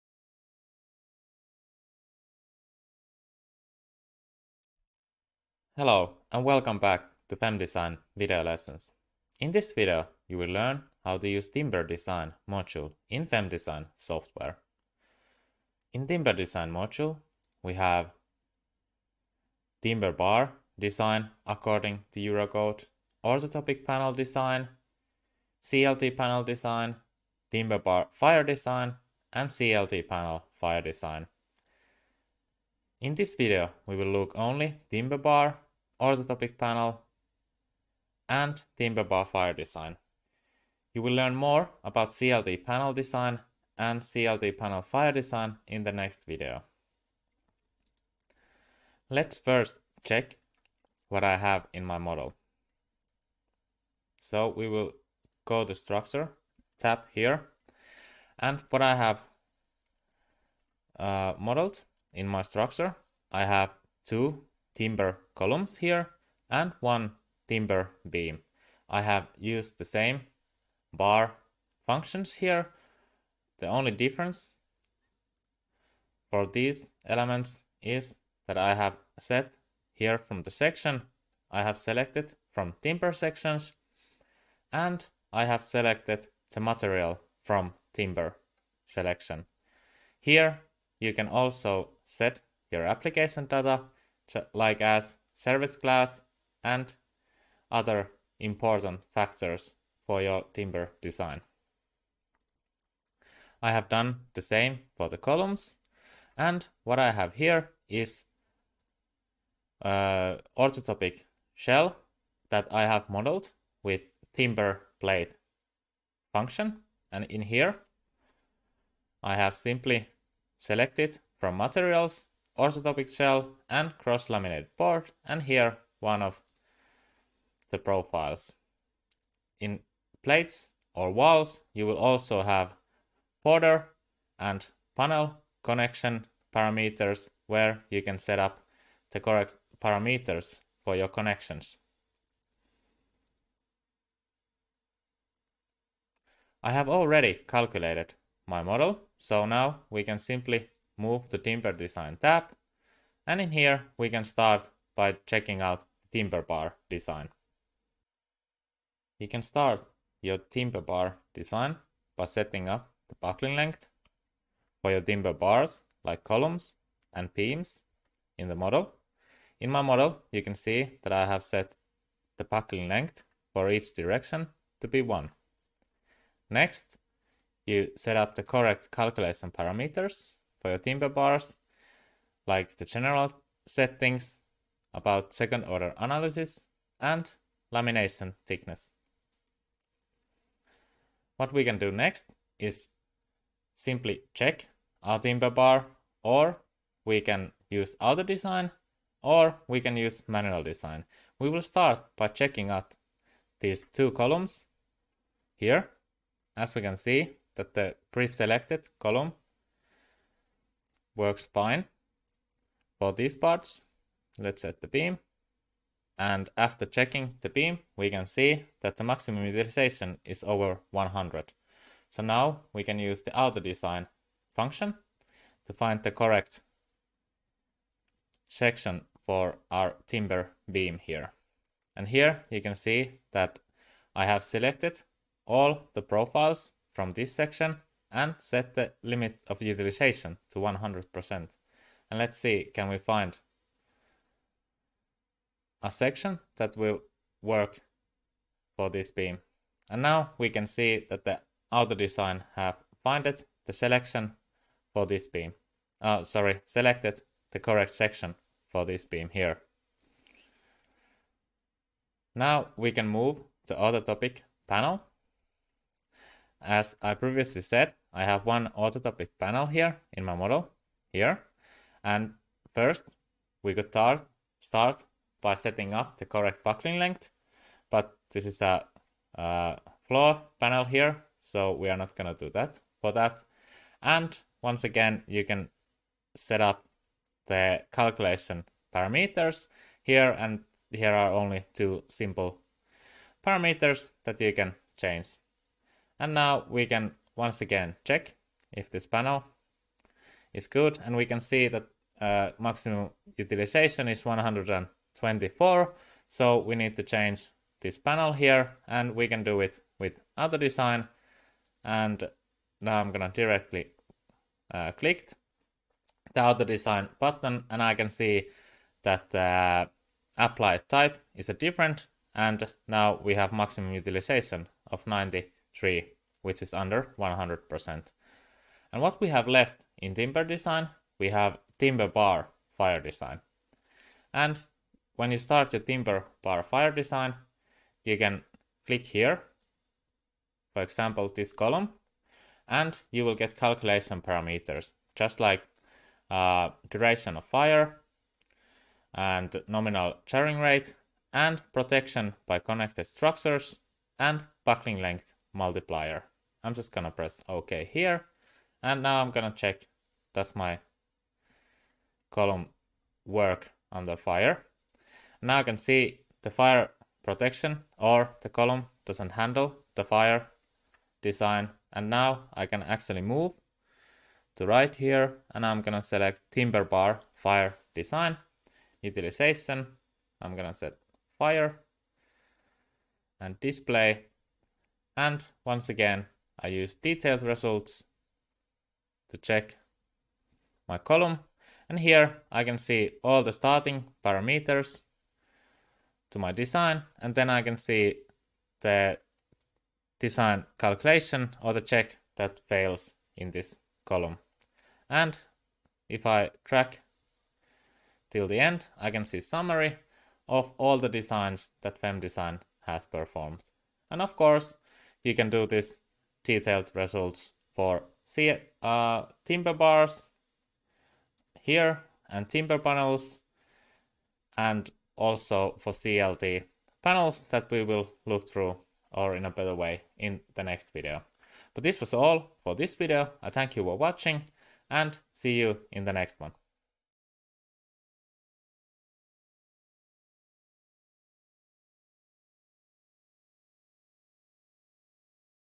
FEM Design User manual: 5.5 Timber design in FEM Design
Watch this short video walkthrough about the timber design module in FEM-Design in order to work optimally with our software for best experience and results. In this video, you will learn more about timber bar design, orthotropic panel, and timber bar fire design.